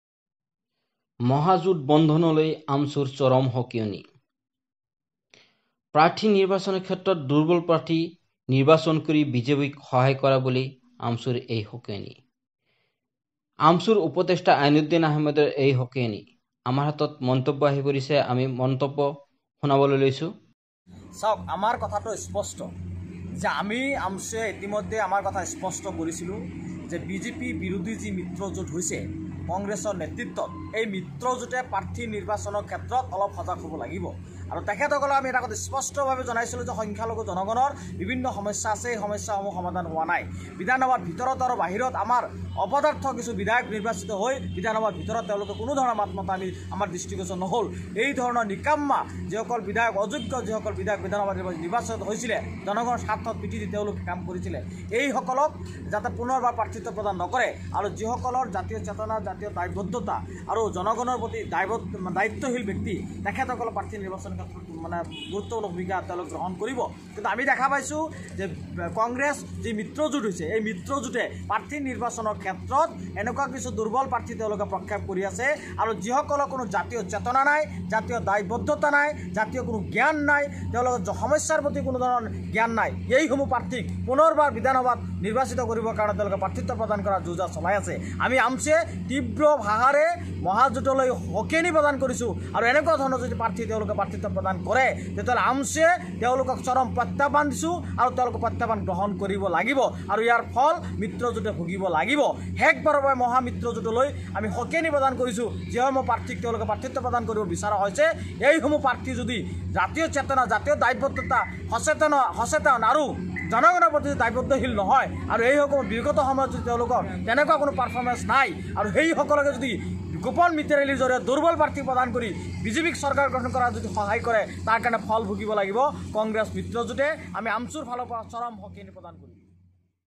মহাজোঁট বন্ধনলৈ চৰম সকীয়নি--আমছুৰ।
প্ৰাৰ্থী নিৰ্বাচনৰ ক্ষেত্ৰত দুৰ্বল প্ৰাৰ্থী নিৰ্বাচন কৰি বিজেপিক সহায় কৰা বুলি আমছুৰ এই চৰম সকীয়নি।